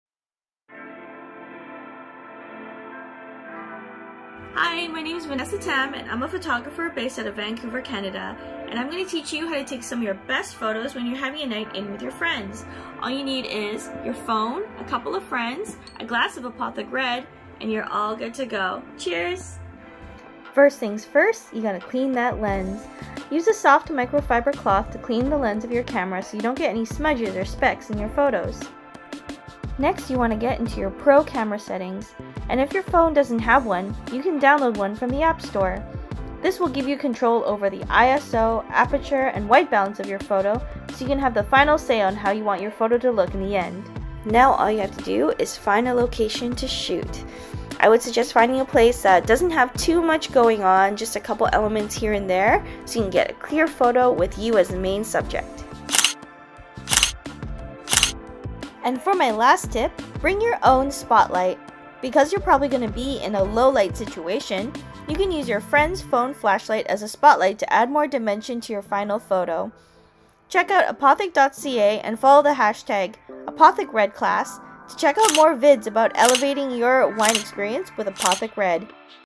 Apothic Red Class - Picture Perfect | Apothic Wines
Mark the night with incredible photos, courtesy of Vanessa. This Vancouver-based photographer will teach you how to take professional-grade photographs with just your smartphone. All you have to do is make it a night worth remembering.